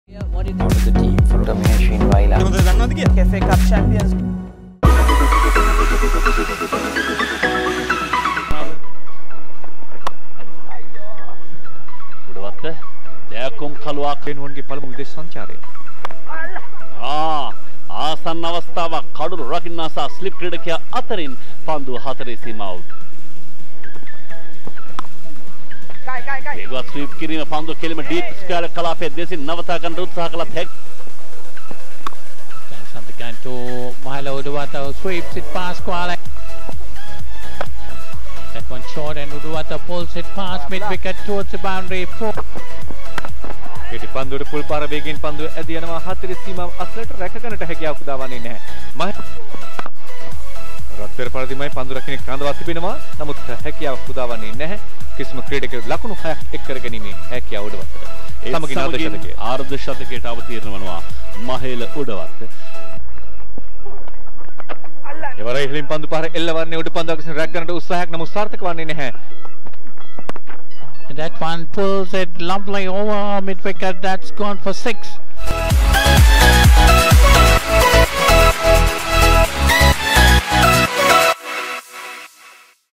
Mahela Udawatte scores 74 in 71 balls
Kandy top order batsman Mahela Udawatte slammed a solid 74 off 71 balls with 7 fours and 2 sixes against Colombo in their SLC Super Provincial Limited Overs Tournament 2018 match at R. Premadasa International Stadium.